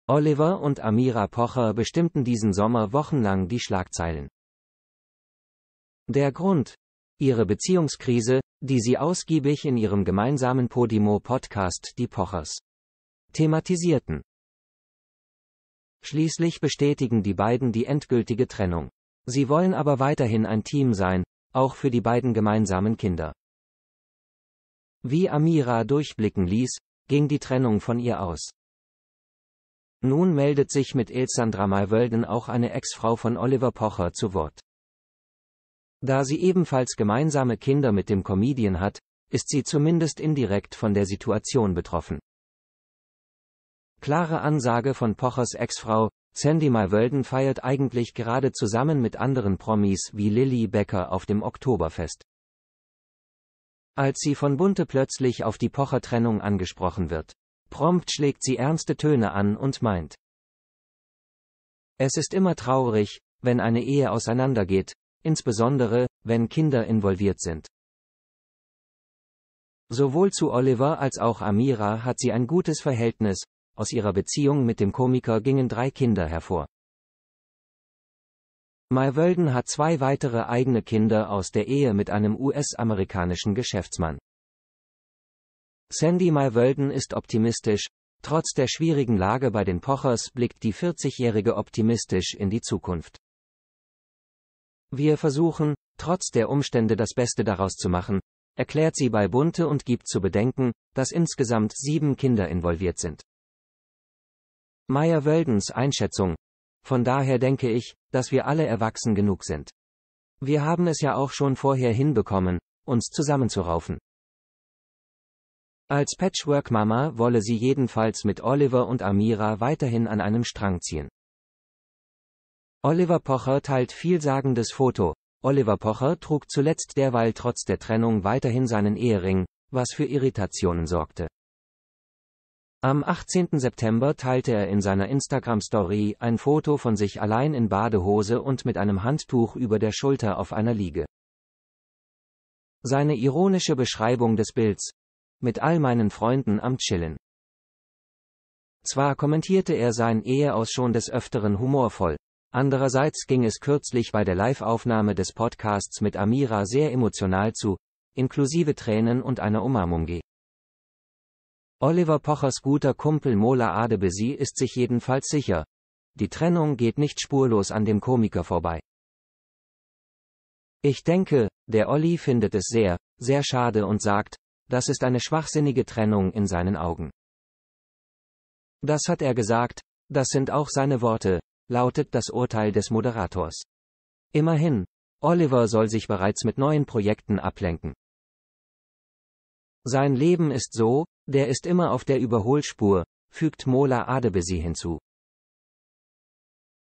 Alessandra Meyer-Wölden äußerte sich zur Scheidung von Oliver Pocher und Amira Pocher
Oliver und Amira Pocher bestimmten diesen Sommer wochenlang die Schlagzeilen.